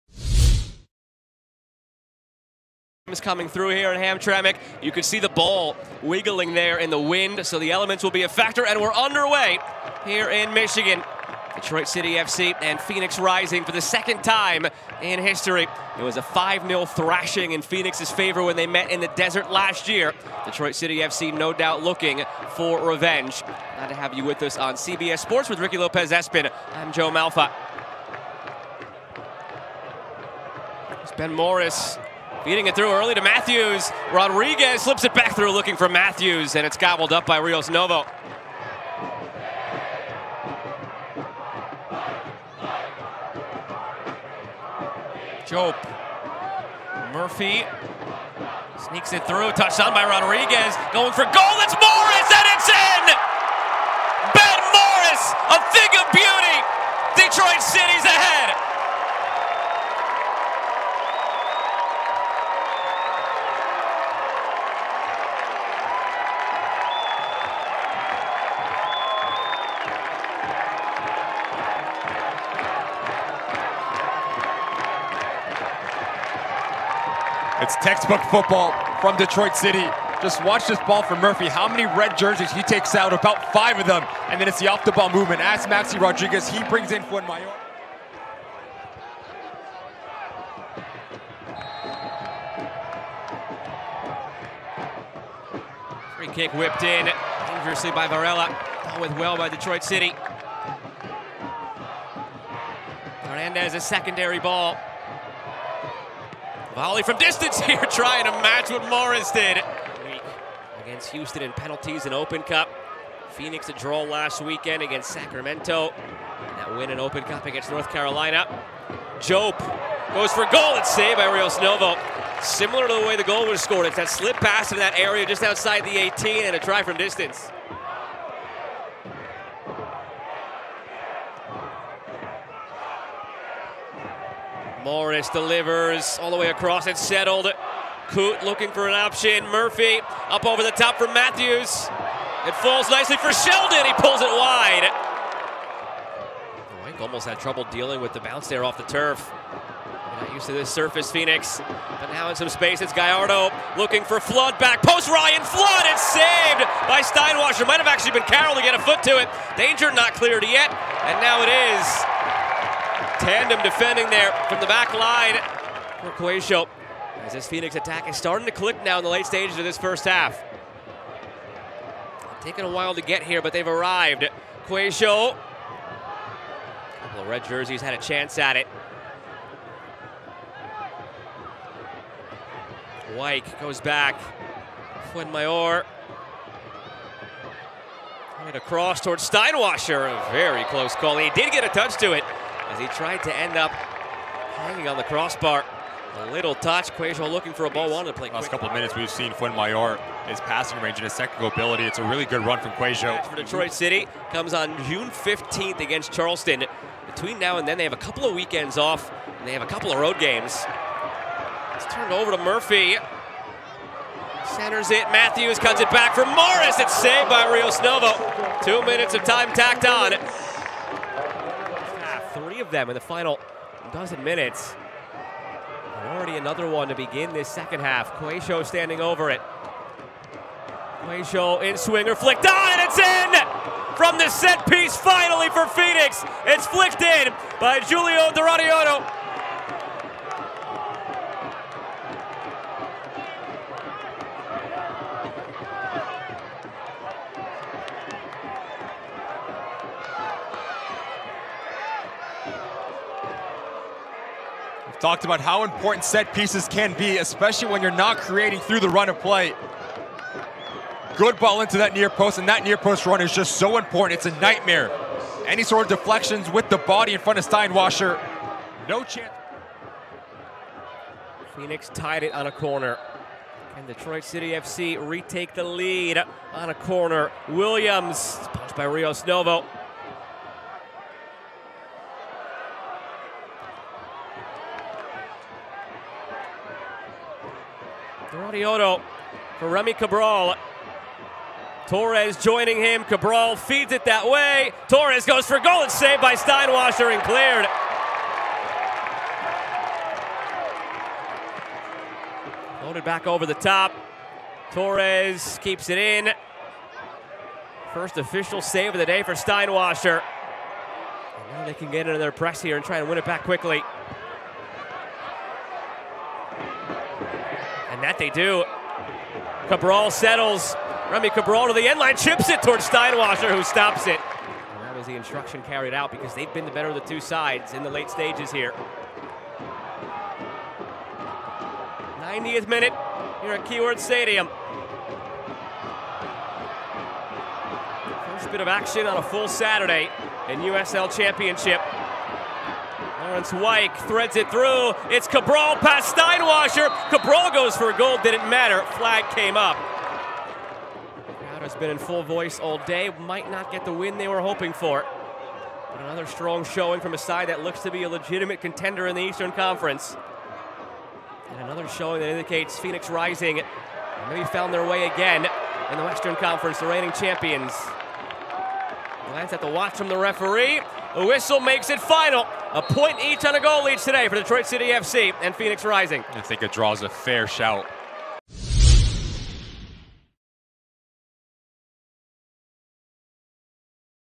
5.11.2024 | Detroit City FC vs. Phoenix Rising FC - Game Highlights
Guilio Doratiotto scored an early second-half equalizer as Phoenix Rising FC earned a 1-1 draw against Detroit City FC at Keyworth Stadium, ending Le Rouge’s perfect home record this season after Ben Morris’ wonderstrike had given the hosts the lead.

00:00:00 - Kick Off by Detroit City FC
00:00:15 - Shot by Maxi Rodriguez
00:00:38 - Goal by Ben Morris
00:01:27 - Cross by Federico Varela
00:01:35 - DefensiveAct by Ben Morris
00:01:45 - Shot by Laurence Wyke
00:01:50 - Pass by James Murphy
00:01:56 - Shot by Abdoulaye Diop
00:02:07 - Cross by Ben Morris
00:02:16 - Shot by Matt Sheldon
00:02:26 - Cross by Erickson Gallardo
00:02:33 - DefensiveAct by James Murphy
00:02:46 - Cross by Emil Cuello
00:02:56 - Pass by Laurence Wyke
00:03:04 - Shot by Emil Cuello
00:03:10 - Defensive act by Brett Levis
00:03:24 - DefensiveAct by James Murphy
00:03:31 - Pass by James Murphy
00:03:35 - Shot by Ben Morris
00:03:43 - Goal by Giulio Doratiotto
00:04:29 - Cross by Ryan Williams
00:04:41 - Pass by Giulio Doratiotto
00:04:48 - Shot by Gabriel Torres
00:04:58 - Cross by Federico Varela
00:05:06 - Pass by Gabriel Torres
00:05:12 - Shot by Rémi Cabral
00:05:25 - DefensiveAct by John Scearce
00:05:36 - Dribble by Laurence Wyke
00:05:42 - Dribble by Rémi Cabral
00:05:51 - DefensiveAct by Devon Amoo-Mensah
00:06:05 - End Match by Detroit City FC